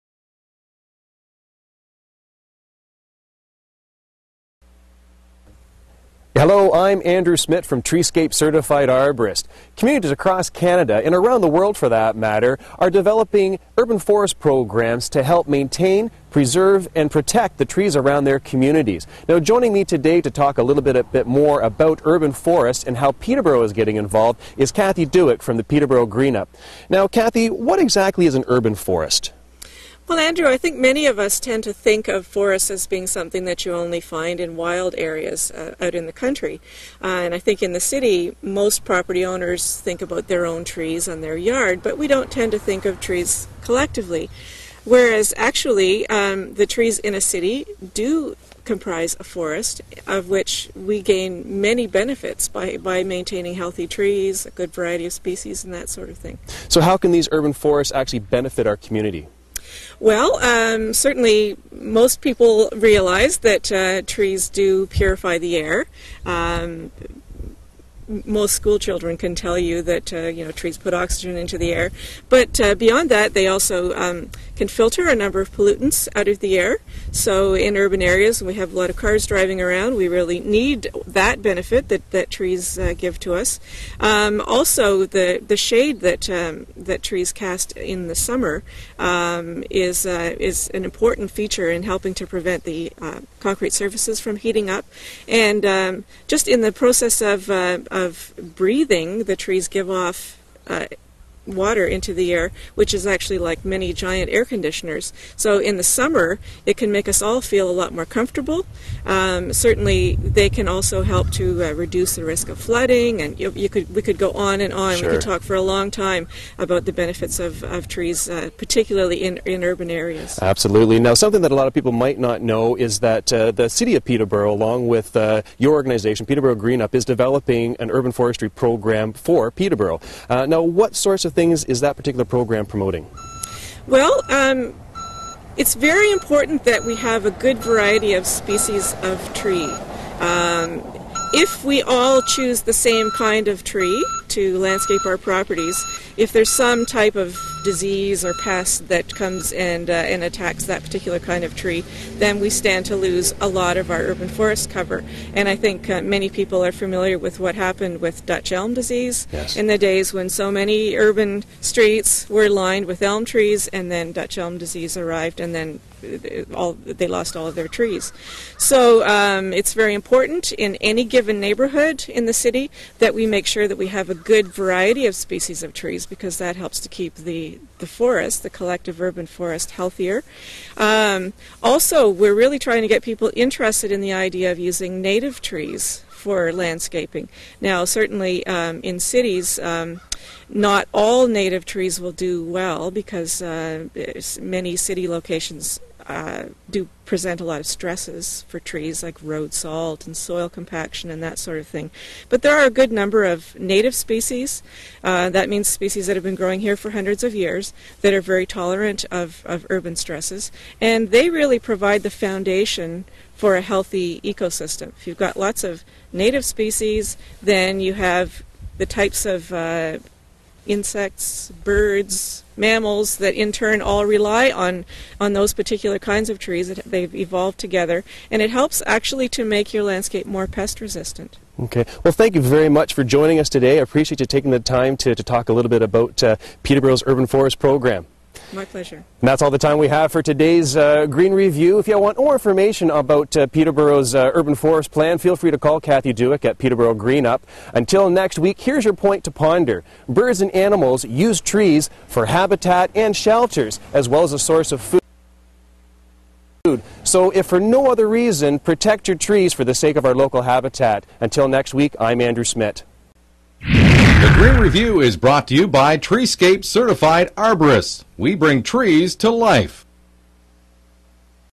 Trees & the Urban Forest
ISA Certified Arborist from Treescape Certified Arborists located in Peterborough Ontario discusses how trees make up our Urban Forest  Key points include; the benefits of trees in an urban environment, the fragile nature of urban forest due to present day environmental stresses.